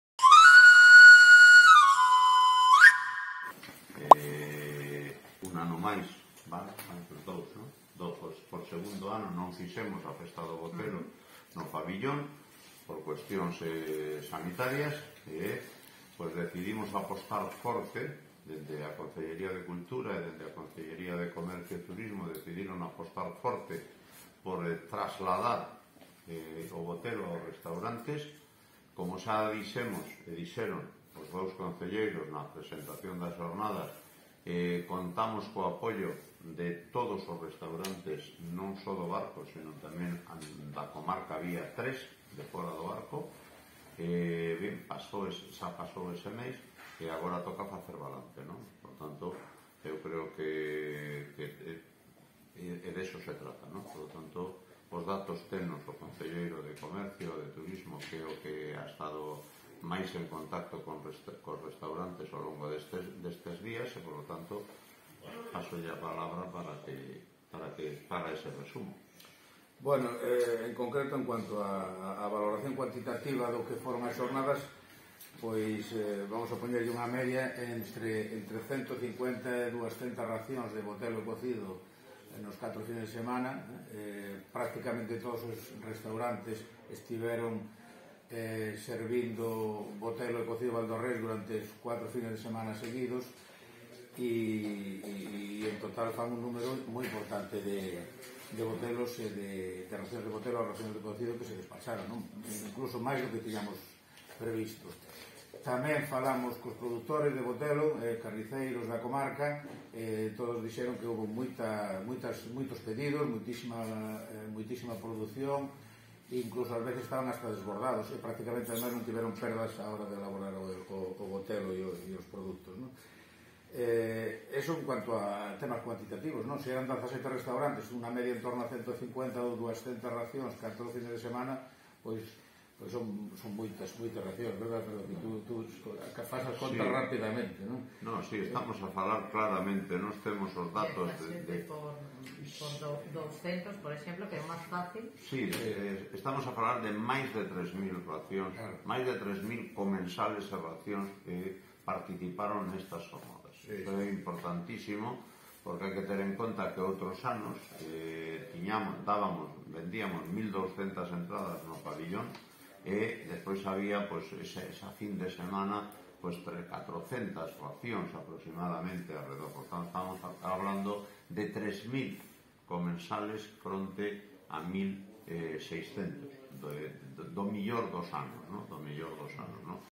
O Barco busca que la Festa do Botelo sea declarada de Interés Turístico Nacional
El Concello de O Barco de Valdeorras trabaja en la solicitud de Fiesta de Interés Turístico Nacional para su Festa do Botelo, que ya posee la declaración autonómica.

Para conseguirlo, pretende llevar a Madrid el embutido tradicional el mes de octubre próximo.

El anuncio fue realizado en la presentación de los resultados de la I Festa de Botelo e Cocido Valdeorrés, en la cual 17 restaurantes de O Barco, Carballeda y Vilamartín sirvieron más de 3.000 menús a otros tantos comensales.

Los números fueron apuntados por el alcalde, Alfredo García, y los concejales de Turismo y Cultura, Eladio Santos y Margarida Pizcueta, respectivamente.